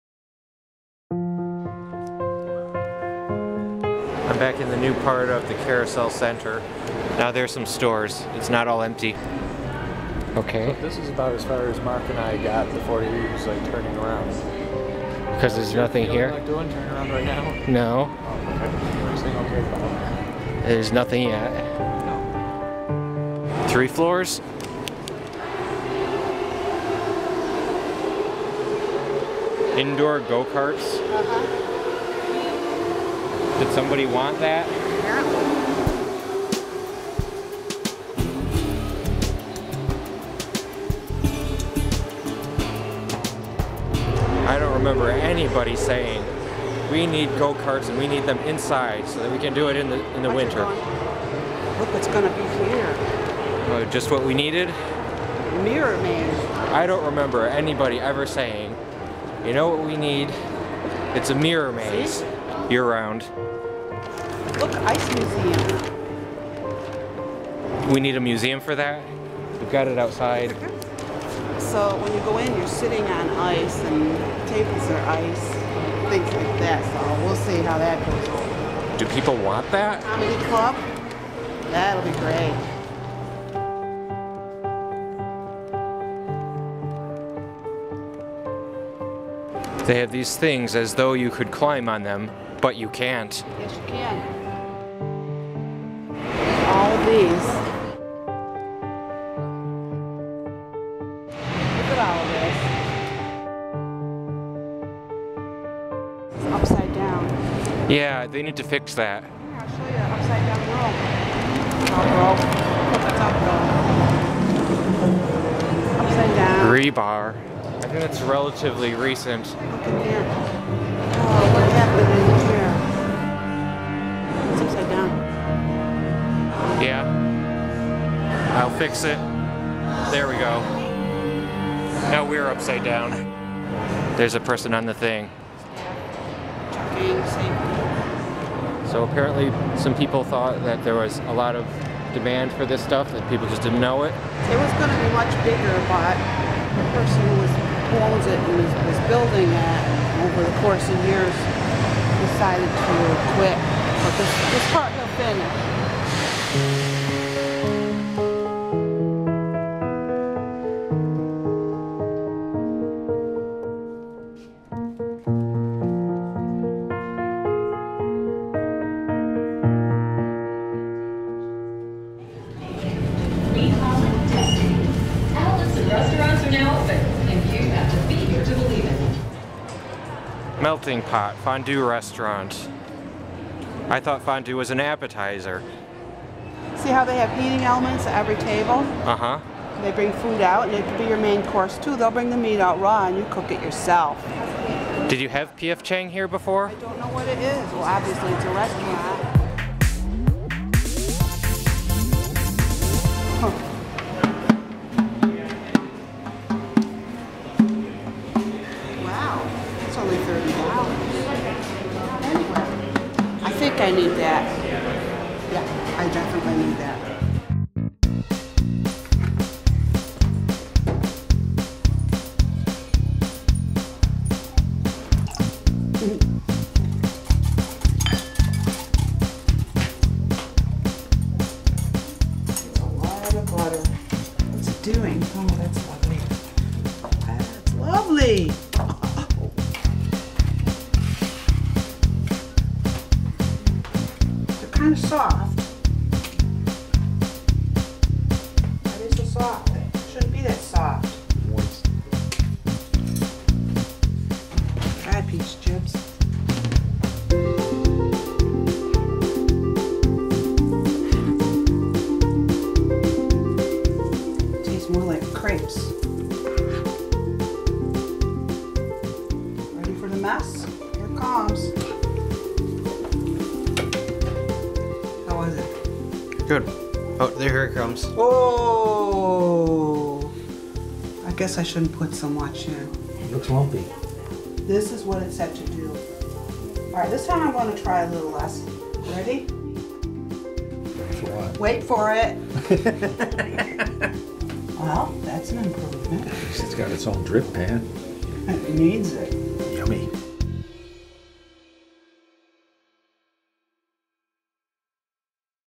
Unnecessary stuff at Carousel Ctr DestiNY, plus waffles! (12/26/2012)
I check out the new businesses at the DestiNY wing of Carousel Center.  Then my mother tries out her new Belgian waffle maker.